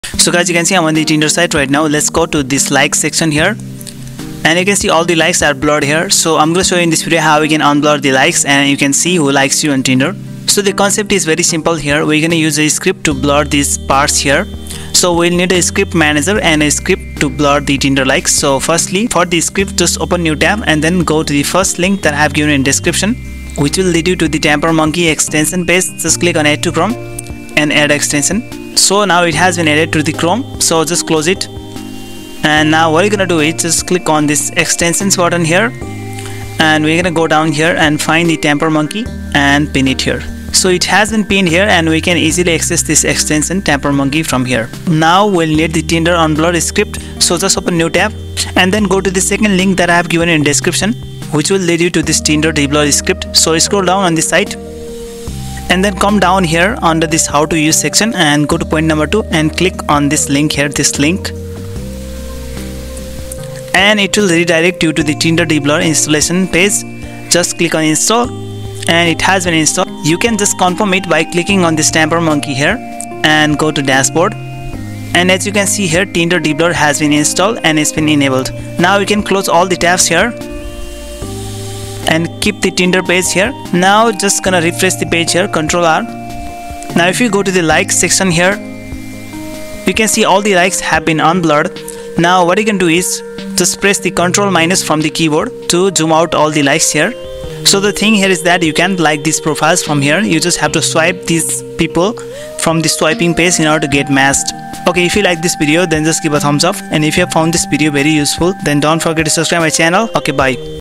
Tinder Unblur Likes | How to see who liked you on Tinder
In this video, i will show you how you can unblur tinder likes. You can unblur likes on Tinder with this method and see who likes you on Tinder.

Topics covered:
- unblur tinder likes
- deblur tinder likes
- tinder unblur